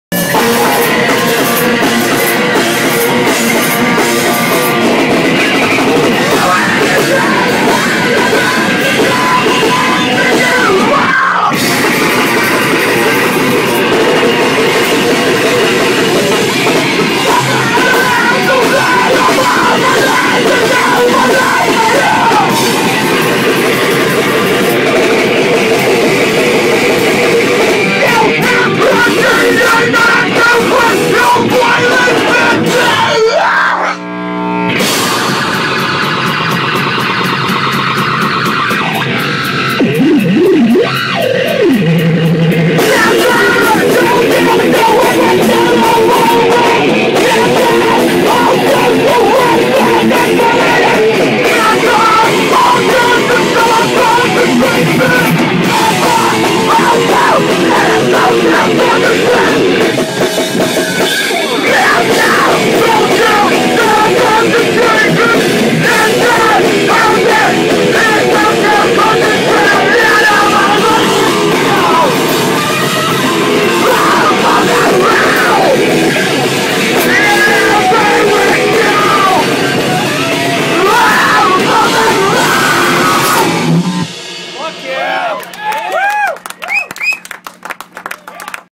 RETOX - LIVE AT BROTHERS LOUNGE
RETOX - LIVE AT BROTHERS LOUNGE - OMAHA, NE - 4.1.14

with:
PLACK BLAGUE
BOX BLONDE